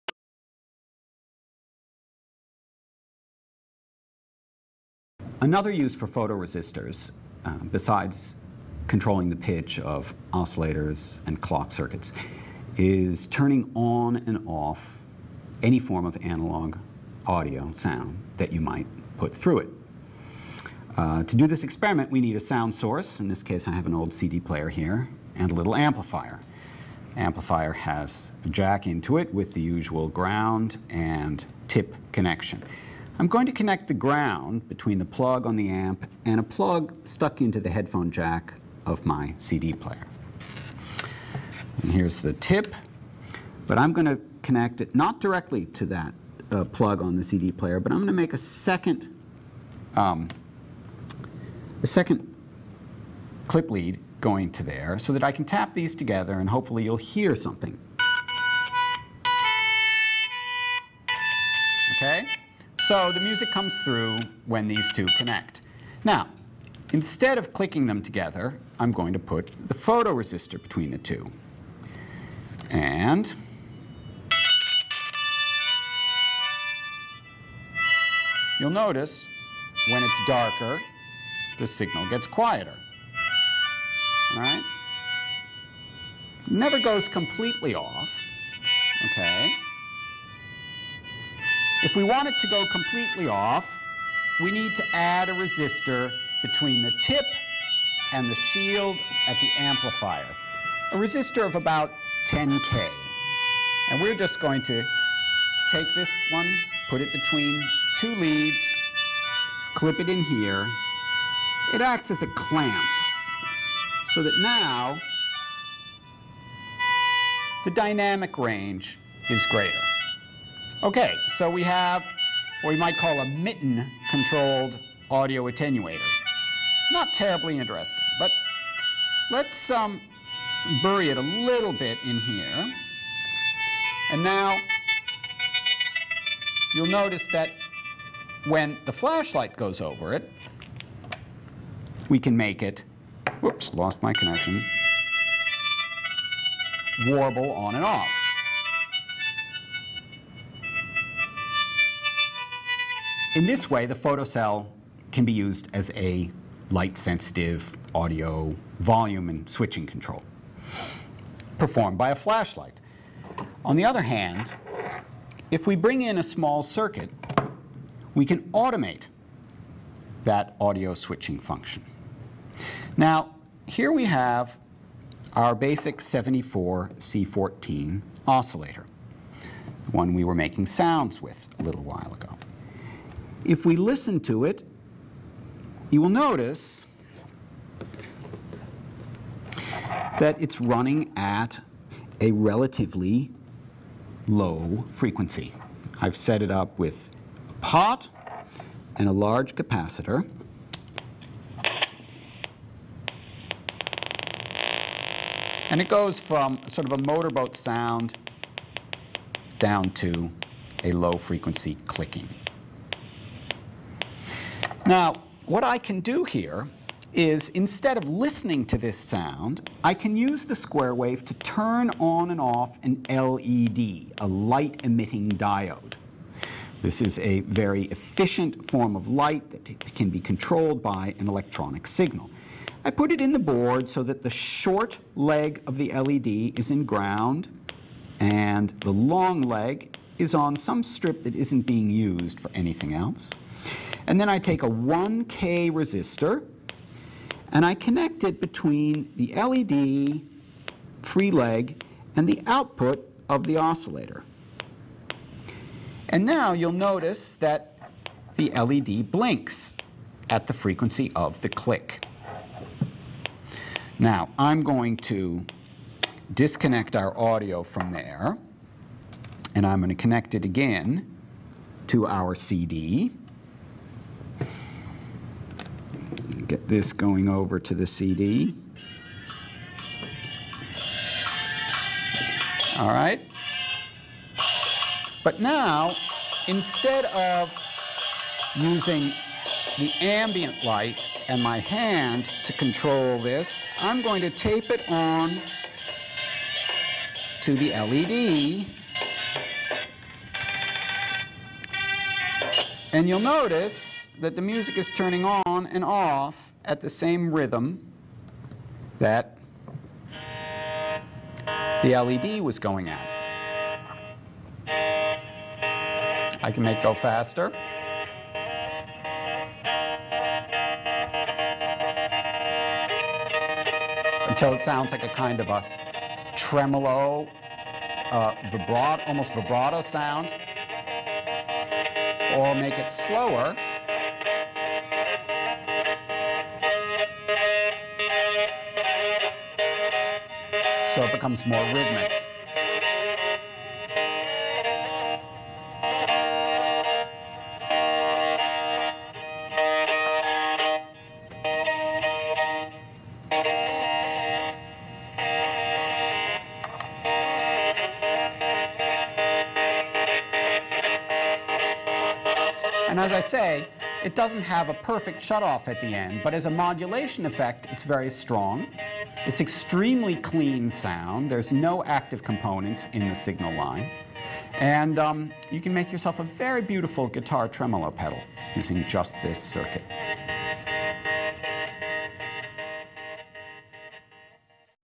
Hack of the Month Club -- Project #13: Tremolos, Gates & Panners
Nic Collins shows you how to use LEDs and photoresistors to make sweet tremolos, gates and panners. From the DVD in "Handmade Electronic Music -- The Art of Hardware Hacking" (Routledge 2009).